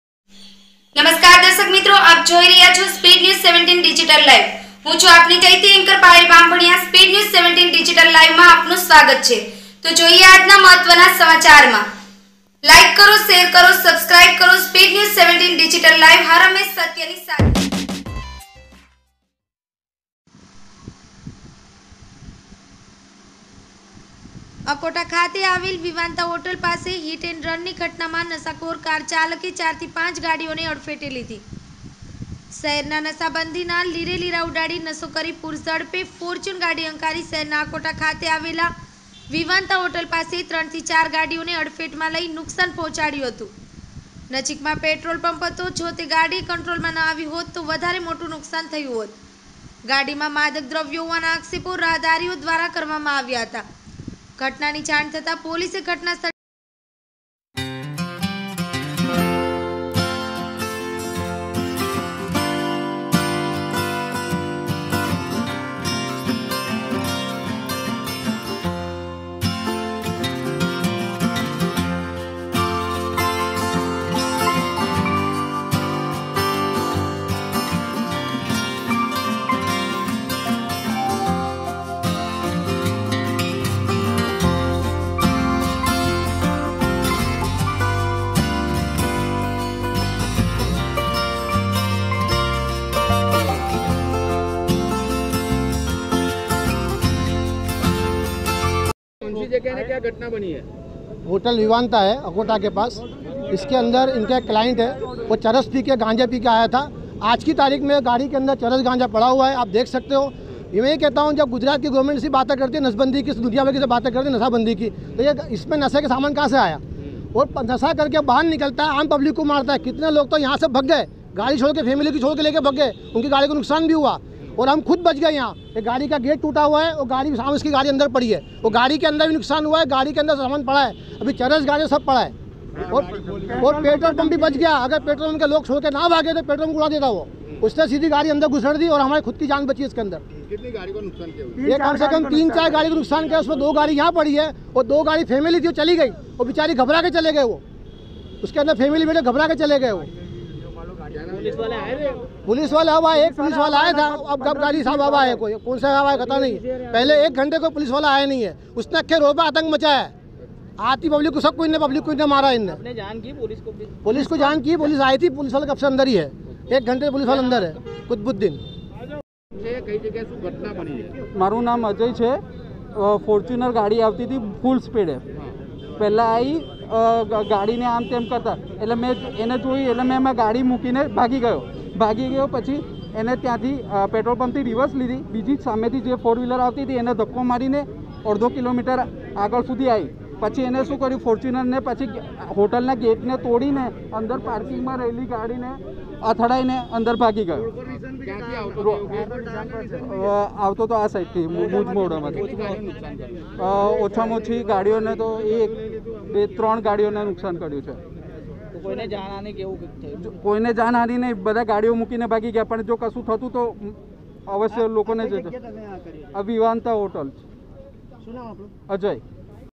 VADODARA અકોટા ખાતે આવેલ વિવાન્તા હોટલ પાસે હીટ એન્ડ રનની ઘટનામાં નશાખોર કારચાલકે ચાર થી પાંચ....
અકોટા ખાતે આવેલ વિવાન્તા હોટલ પાસે હીટ એન્ડ રનની ઘટનામાં નશાખોર કારચાલકે ચાર થી પાંચ ગાડીઓને અડફેટે લીધી

શહેરમાં નશાબંધીના લીરેલીરા ઉડાડી નશો કરી પૂરઝડપે ફોર્ચ્યુનરગાડી હંકારી શહેરના અકોટા ખાતે આવેલા વિવાન્તા હોટલ પાસે ત્રણ થી ચાર ગાડીઓને અડફેટમાં લ ઇ નુકસાન પહોંચાડ્યું હતું નજીકમાં પેટ્રોલપંપ હતો જો તે ગાડી કંટ્રોલમાં ન આવી હોત તો વધારે મોટું નુકશાન થયું હોત ગાડીમાં માદક દ્રવ્યો હોવાના આક્ષેપો રાહદારીઓ દ્વારા કરવામાં આવ્યા હતા ઘટનાની જાણ થતાં પોલીસે ઘટનાસ્થળે પહોંચી કાર્યવાહી હાથ ધરી હતી

નઝીર શેખ  સાથે  સલીમ મુલતાની વડોદરા